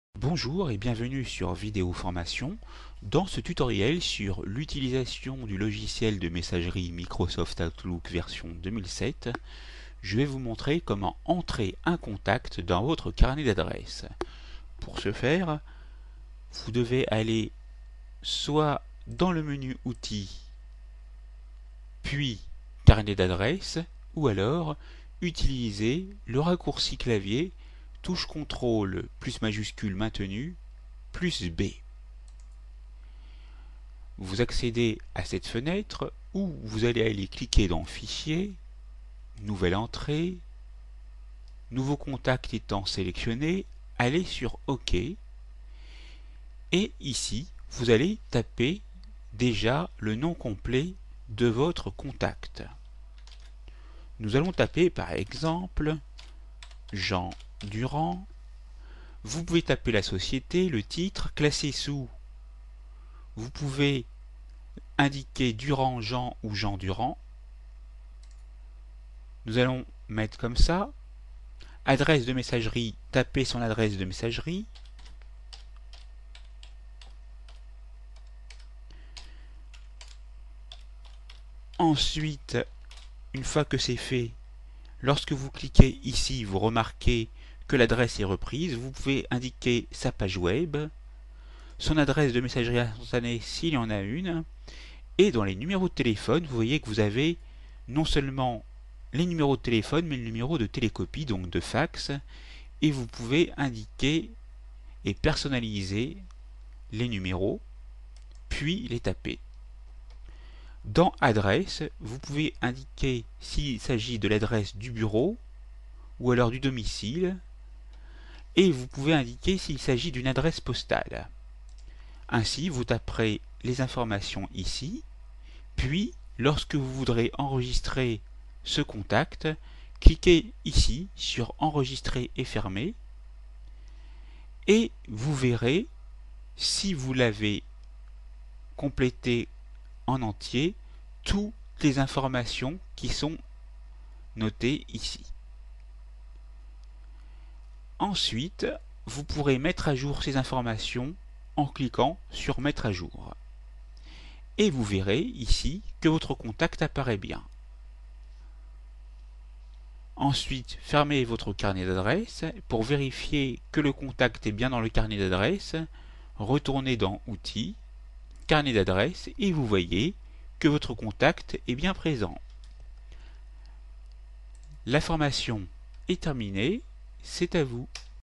Entrer un contact dans votre carnet d'adresses avec Microsoft Office Oulook
Dans ce tutoriel vidéo d'informatique et de bureautique sur le logiciel de messagerie électronique Microsoft Office Outlook, je vais vous montrer comment Entrer un contact dans son carnet d'adresses.